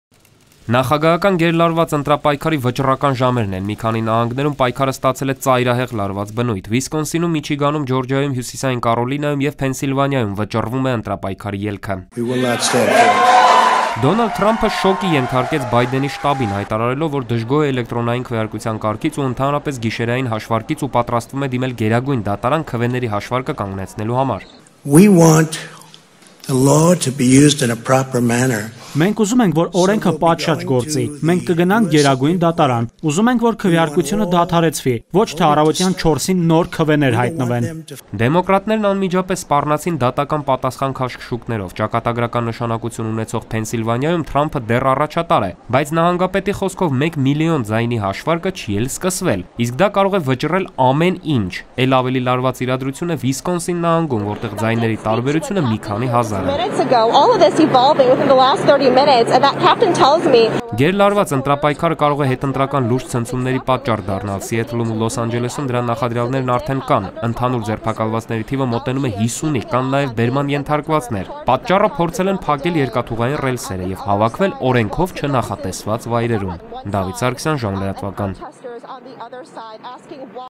ԱՄՆ-ում սպասվում են հետընտրական ցնցումներ, հնարավոր են դատական քաշքշուկներ
Գերլարված ընտրապայքարը կարող է հետընտրական լուրջ ցնցումների պատճառ դառնալ: Սիեթլում ու Լոս Անջելեսում դրա նախադրյալներն արդեն կան: Ընդհանուր ձերբակալվածների թիվը մոտենում է 50-ի, կան նաև բերման ենթարկվածներ: Պատճառը՝ փորձել են փակել երկաթուղային ռելսերը և հավաքվել՝ օրենքով չնախատեսված վայրերում: